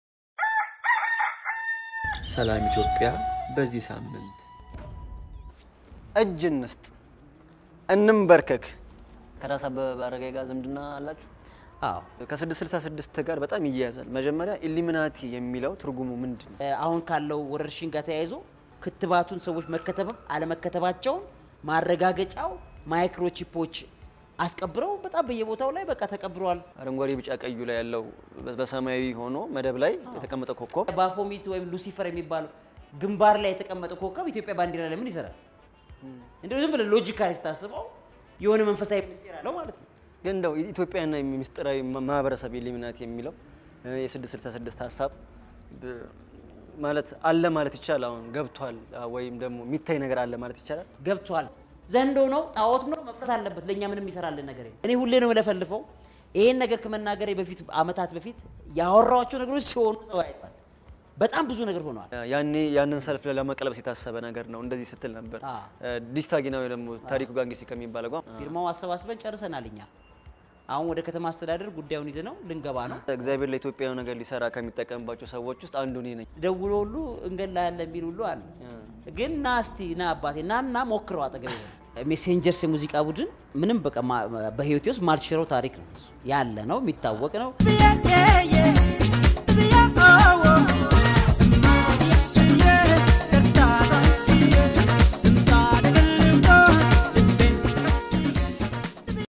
🚦 ሠላም - ኢትዮጵያ በዚህ ሳምንት ከጋዜጠኛ እና መምህር አብይ ይልማ ጋር ተከስቷል | ስለ 666 | ኢትዮጵያ እና ተያያዥ ጉዳዮች ተጫውተናል | ይጠብቁን
በየሳምንቱ የሚለቀቁትን ፕሮግራሞች ለመከታተል ቤተሰብ ይሁኑን |